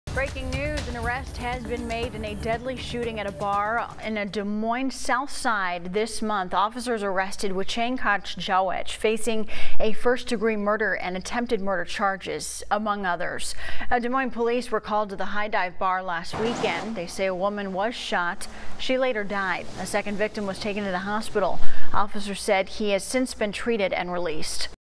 Des Moines resident arrested in killing of Omaha woman, police say
Police arrested Wichang Gach Chawech on First Degree Murder and several other charges for the fatal shooting of Nyamal Deng.